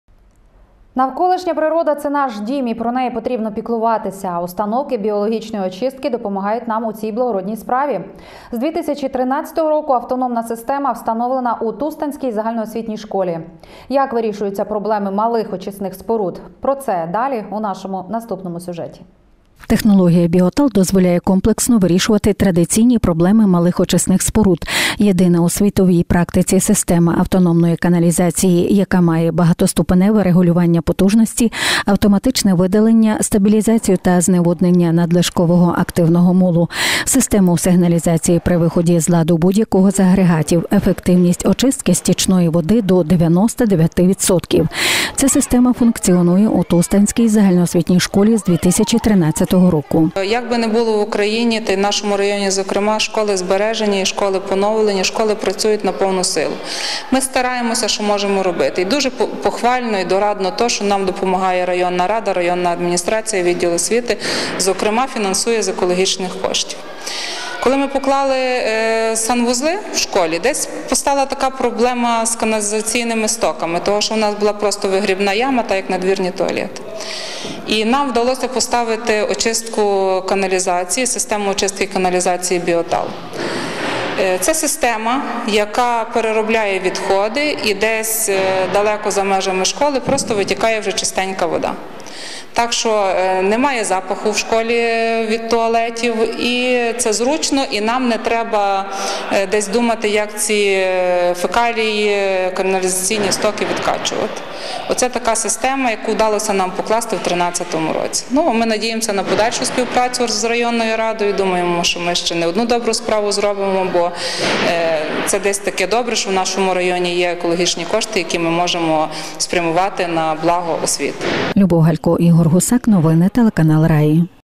Як вирішуються проблеми малих очисних споруд?
Навколишня природа - це наш дім, і про неї потрібно піклуватися, а установки біологічної очистки допомагають нам у цій благородній справі. З 2013 року автономна система встановлена у Тустанській ЗОШ. Як вирішуються проблеми малих очисних споруд? Про це далі у нашому наступному сюжеті.